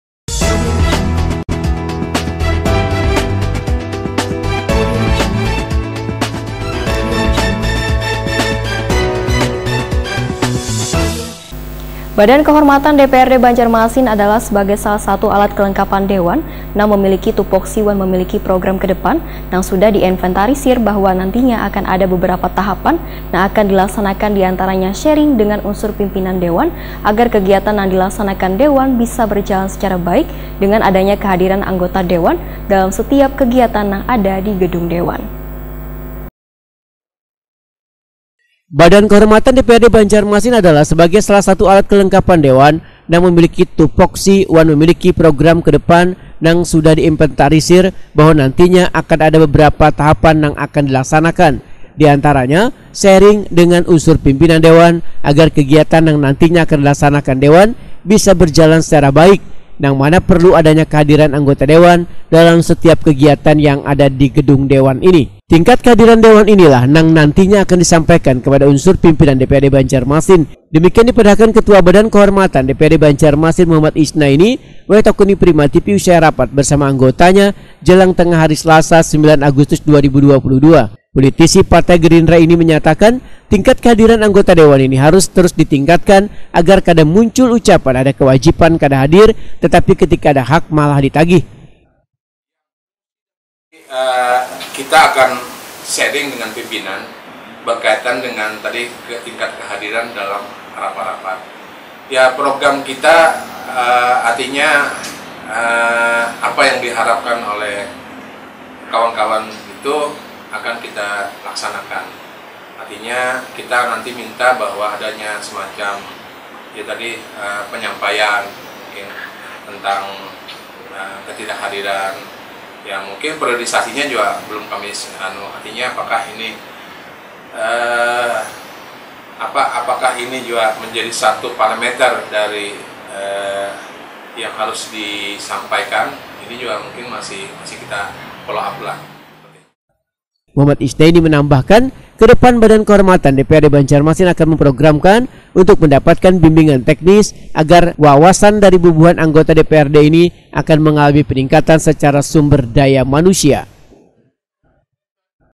TINGKAT KEHADIRAN ANGGOTA DEWAN INI HARUS TERUS DITINGKATKAN
Badan Kehormatan DPRD Banjarmasin adalah sebagai salah satu alat kelengkapan dewan nang memiliki tupoksi wan memiliki program kedepan nang sudah di inventarisir bahwa nantinya akan ada beberapa tahapan nang akan dilaksanakan diantaranya sharing dengan unsur pimpinan dewan agar kegiatan nang nantinya akan dilaksanakan dewan bisa berjalan secara baik nang mana perlu adanya kehadiran anggota dewan dalam setiap kegiatan nang ada di gedung dewan ini//